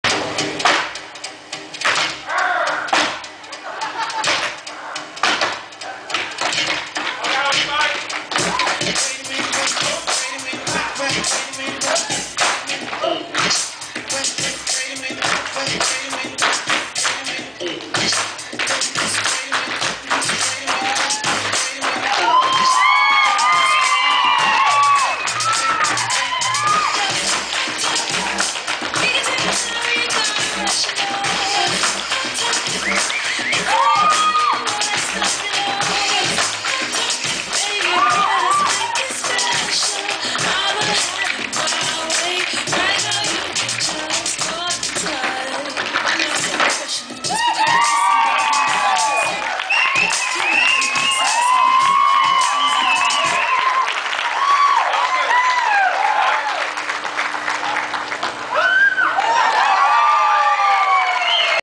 Hot shot tap dance at the Deschutes county fair
We had learned this dance in a workshop the day before in like 45 mins. Then we got to go do it. I had so much fun these guys are AMAZING!!!